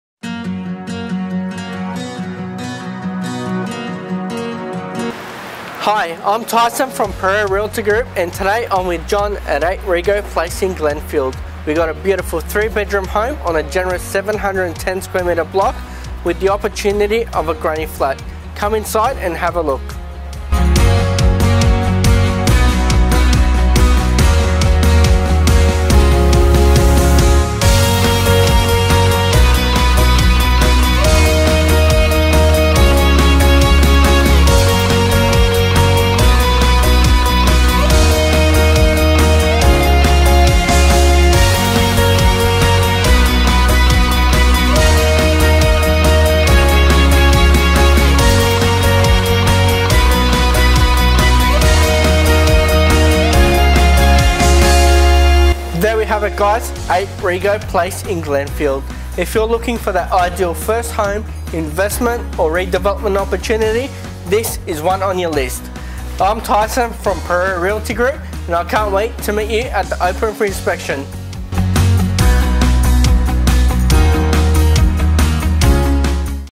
PRG 8 Rigo Place Glenfield
This brick veneer home sits on a massive 710 m2 block of land in a very quiet cul-de-sac in the sought after suburb of Glenfield.

The home presents exciting scope to modernise or rebuild and enjoys close proximity to the ever popular Glenfield train station, a very modern shopping complex with a variety of retail shops and medical centres.

All bedrooms have built ins and ceiling fans and there is a good sized lounge and dining with a kitchen that can be updated and a modern bathroom and separate toilet.

Adjoining the kitchen is a fairly sized outdoor area that is ideal for entertaining overlooking the large rear yard. There is a side access that leads to the single carport and this could suit a handyman or tradie.

This is a great opportunity to enter into home ownership if you are a first home buyer and an equally fantastic proposition for the investor wanting to capitalise on a prime area and add a granny flat for dual income (STCA).

This home is currently tenanted at $460 per week.

*Pereira Realty Group believes all information contained herein is accurate at the time of advertising. However, we encourage interested parties to conduct their own enquiries.